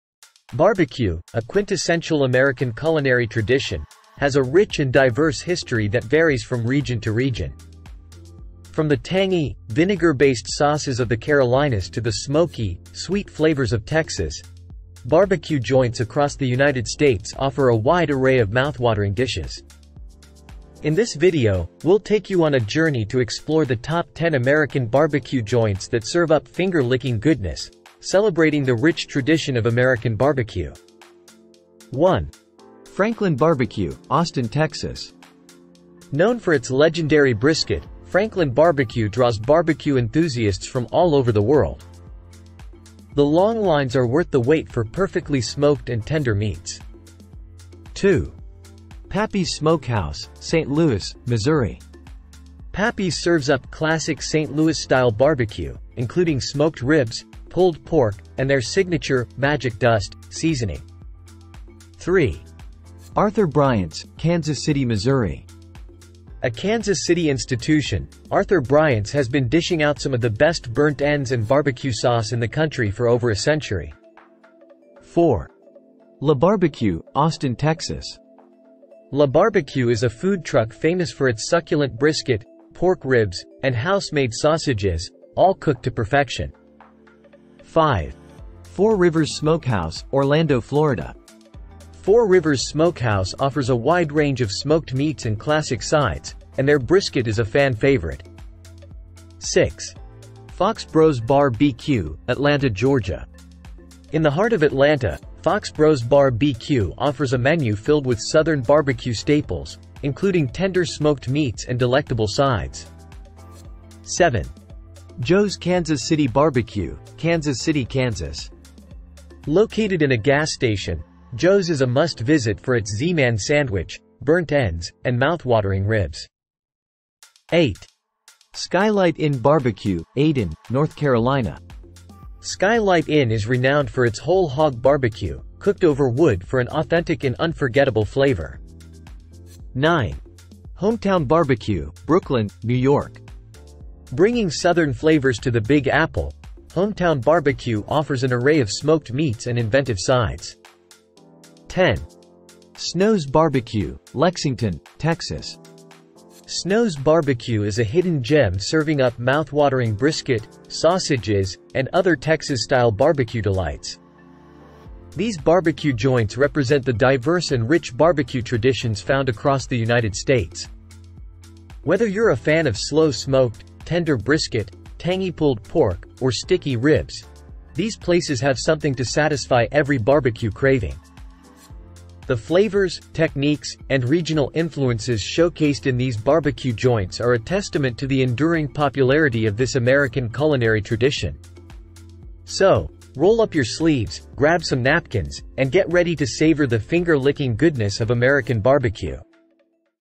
Top 10 American BBQ Joints for Finger Licking Goodness
Get your taste buds ready for a flavorful journey as we explore the top 10 American BBQ joints that serve up finger-licking goodness.